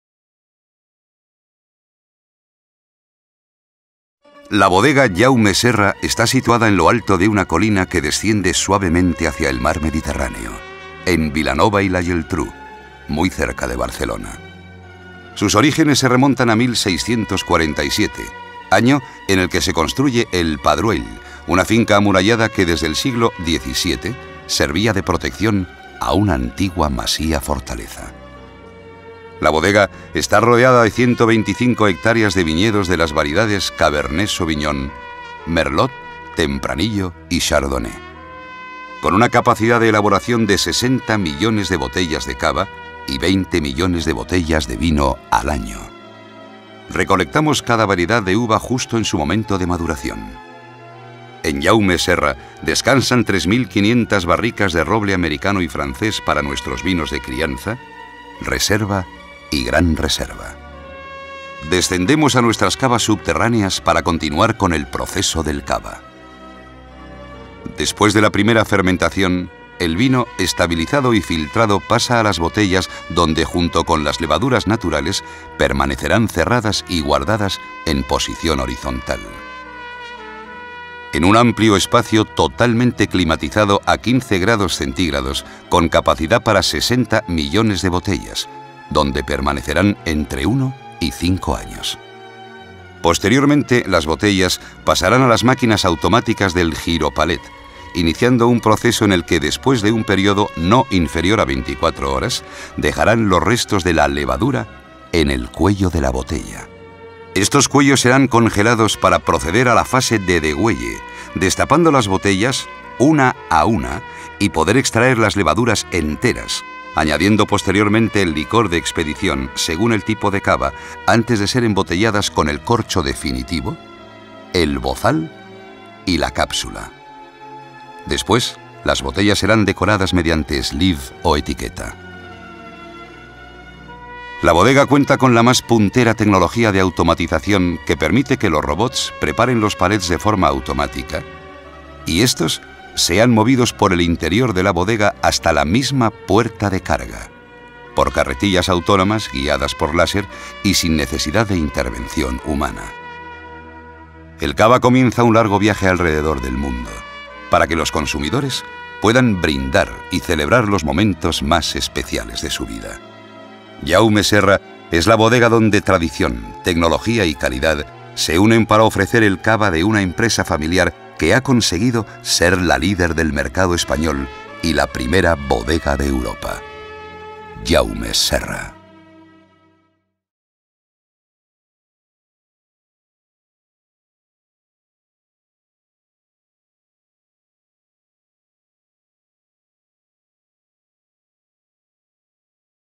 Bodega Jaume Serra
Pasea por la Bodega de la marca líder en Cava, Jaume Serra combina Tradición en la elaboración junto con la máxima tecnología